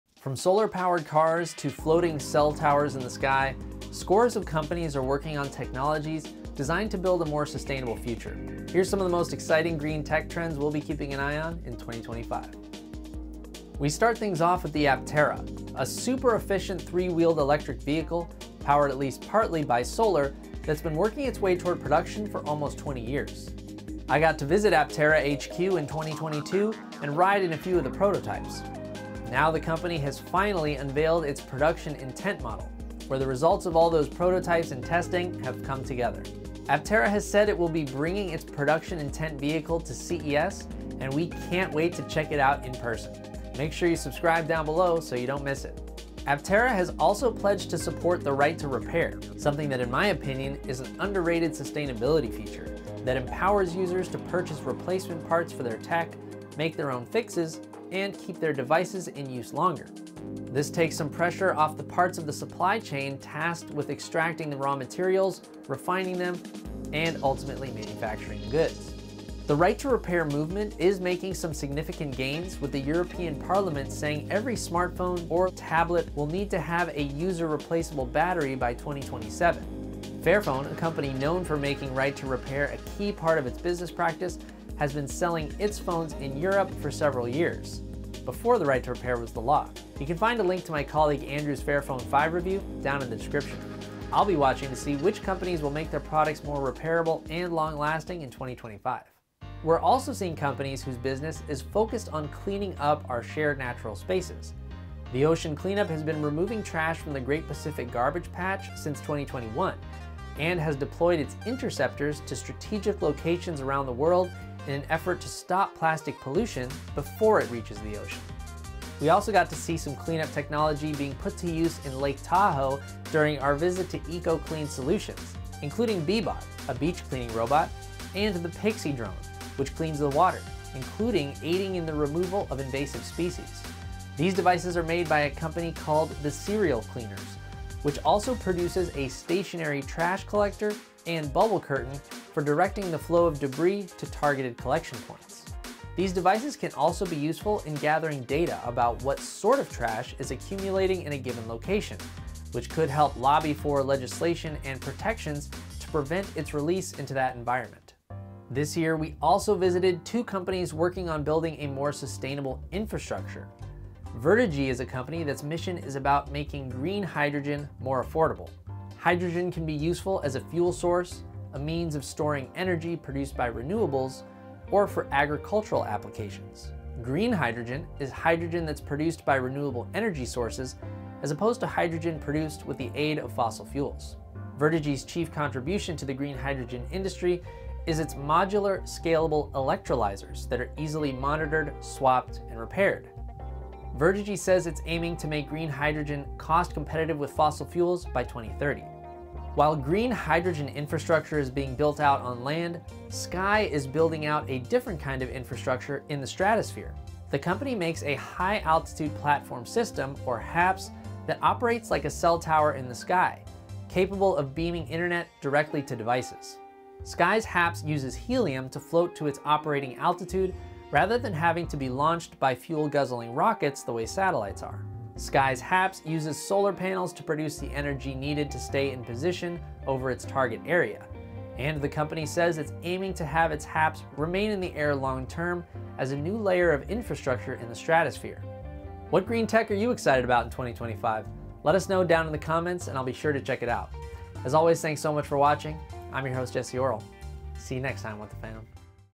Green Tech to Watch in 2025
From solar powered cars to floating cell towers in the sky, we'll be running down our list of the top sustainable tech trends to watch in 2025.

Read the CNET Article: Sustainable Tech Trends to Watch in 2025

Read the Fairphone 5 Review: The Phone That Wants to Save the World

0:00 Intro
0:14 Aptera Solar Powered Car
1:10 Right To Repair
1:21 Fairphone 5
1:39 The Ocean Cleanup
1:59 Eco-Clean Solutions Cleaning Robots
2:15 Searial Cleaners
2:39 Verdagy Green Hydrogen
3:23 Sceye High Altitude Platform System